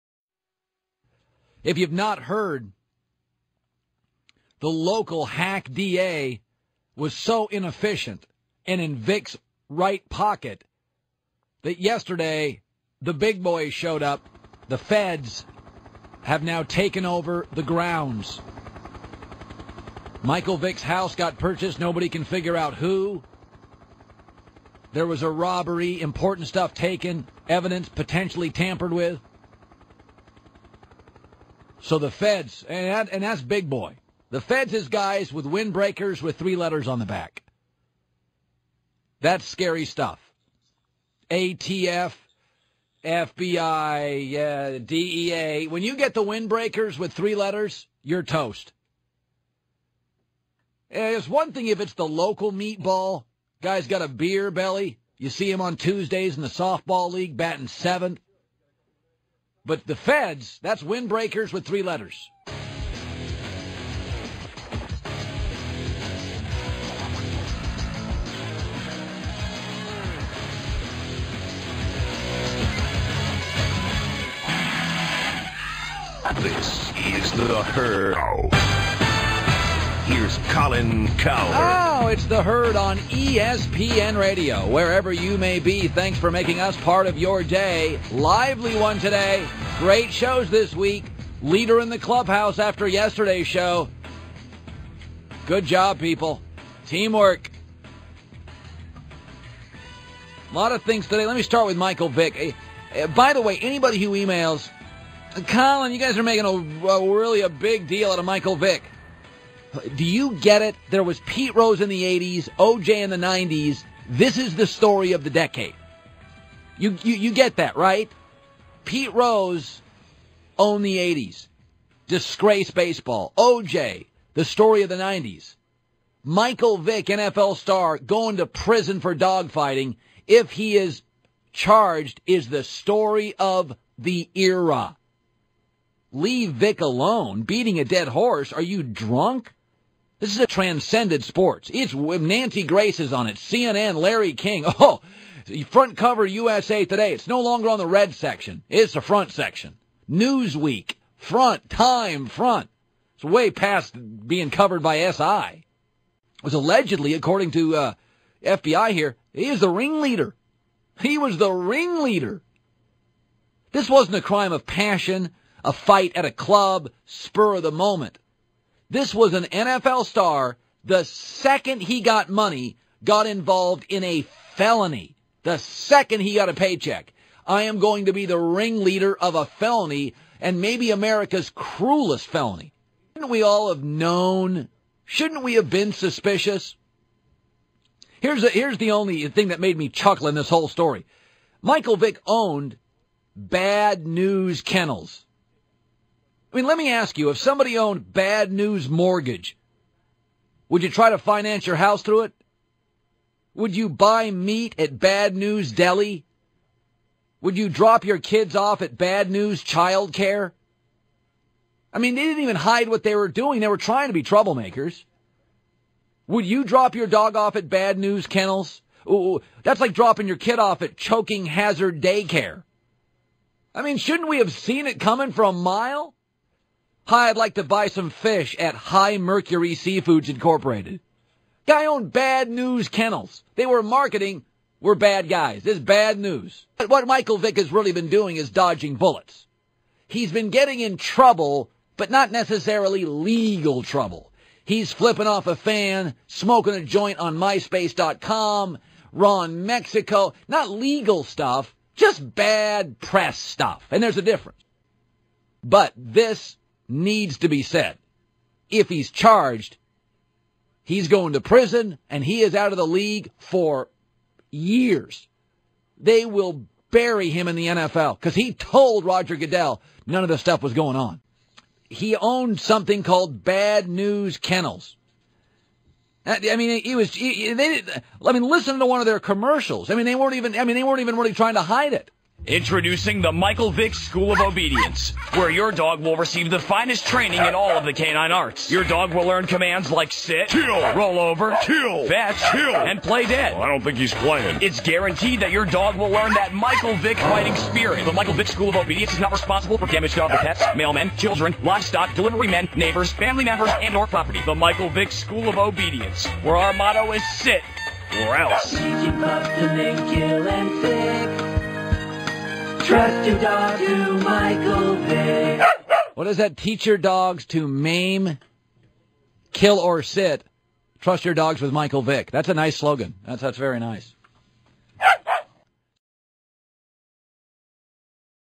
Colin Cowherd - Mike Vick Extravaganza
I had a idea to do a Mike Vick video before, but I did different ones instead. I thought it was about time to finally make a new video and I revisited that idea and I decided to splice together a whole bunch of clips from various shows to create this Mike Vick extravaganza. You might be able to tell, but it still is pretty seamless. It's not as straightforward as the ones from before, but I like that as thus it's different. Anyways there were a couple soundbytes I remembered that I was unable to find, namely one about Keith Brooking where he was praising Mike Vick for showing up at every camp (that's like saying you deserve credit for taking care of your kids). There are a ton of frames in this video though. The NFL is just around the corner (pre-season has started) and I can't wait.